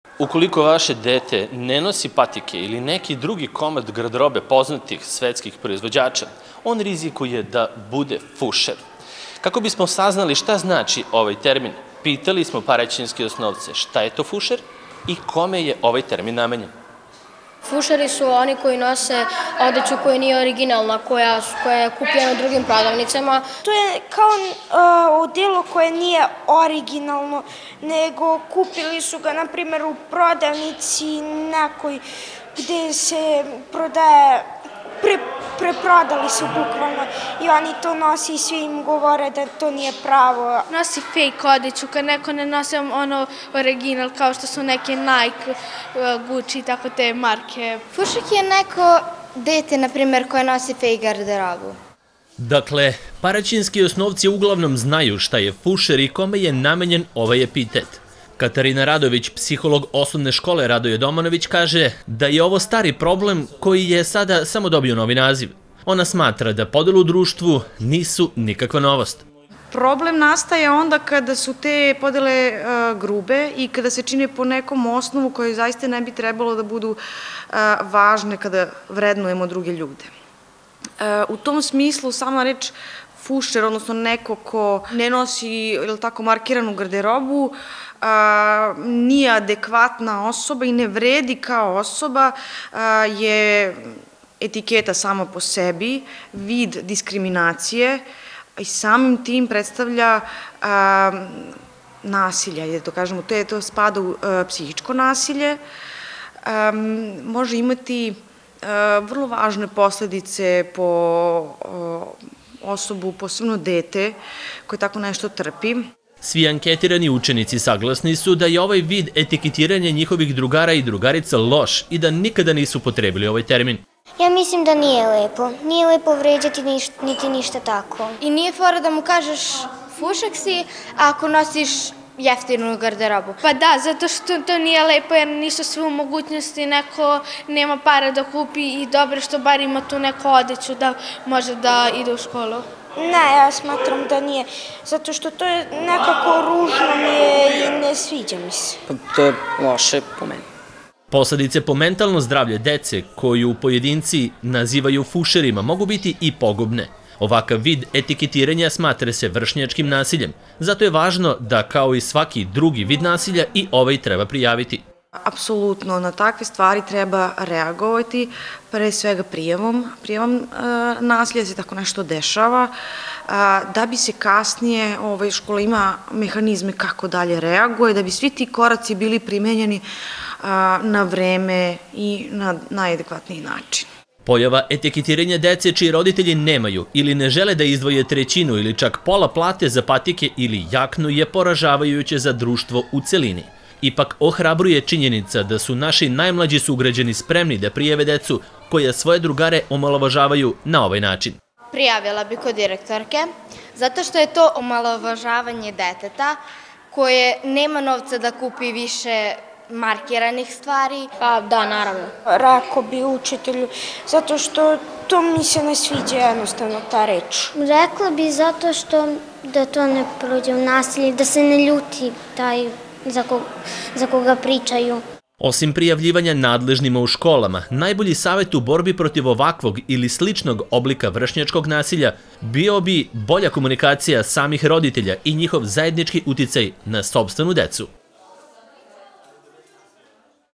Šta u današnjem jeziku mladih znači biti “fušer”?
Novi oblik vršnjačkog nasilja u školama predstavlja segragacija učenika na osnovu garderobe koju nose. Sve češće se u školama pominje termin “fušer” koji kako su nam objasnili paraćinski osnovci, predstavlja termin namenjen deci koja ne nose firmiranu odeću i obuću. Šta o ovoj temi kažu učenici osnovnih škola u Paraćinu a šta im poručuju psiholozi?